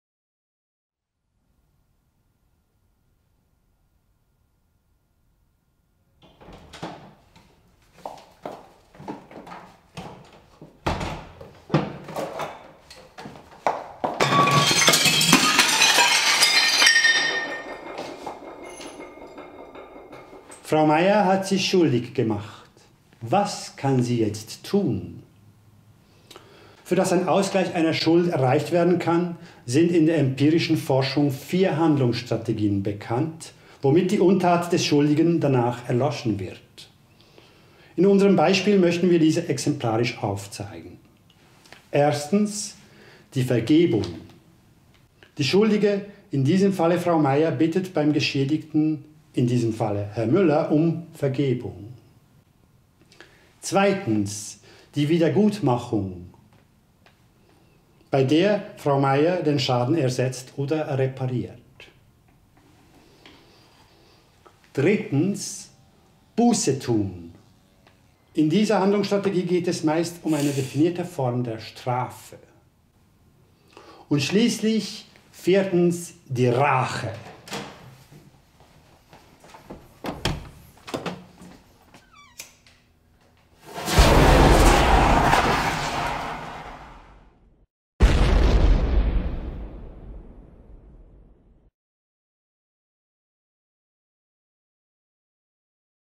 Die zerbrochene Vase
Schuldig?
Prof. Dr phil. hist. Peter A. Hornstein zeigt Ihnen Ihre Möglichkeiten.

Eine Produktion von ABER! zum Thema "schuldig" im Rahmen des ZFF72 2018 - hier die leicht längere Version.

Von Vera Liechti, Stefan Käsermann & Corina Spaeth

mit Corina Spaeth als Frau Meier
       Stefan Käsermann als Herr Müller
       János Gazdag als Prof. Dr phil. hist. Peter A. Hornstein